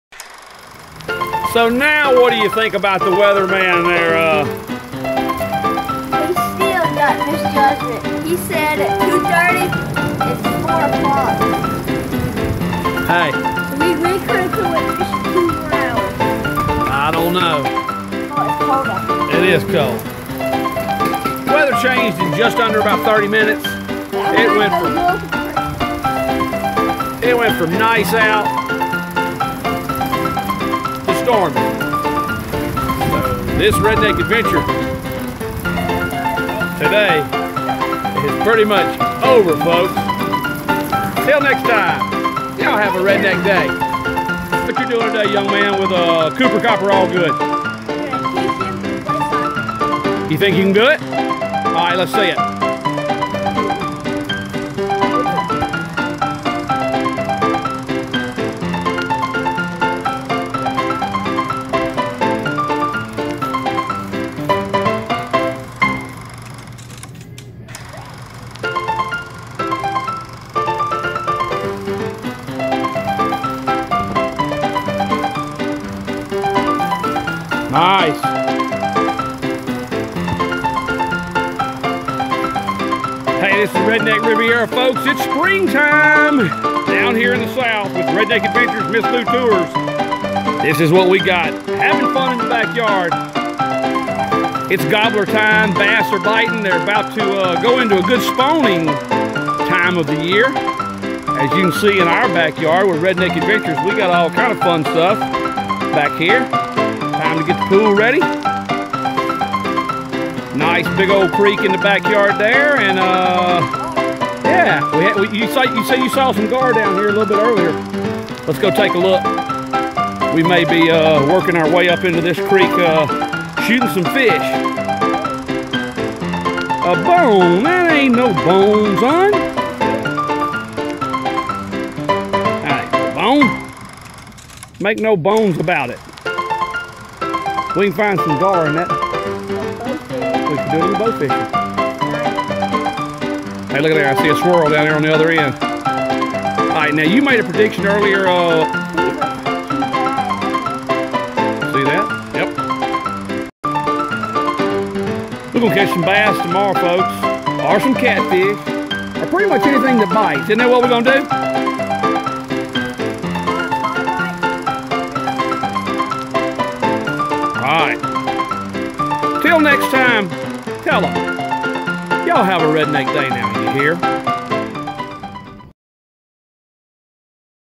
Redneck Rainout
Backyard fun shooting Gars and a rainout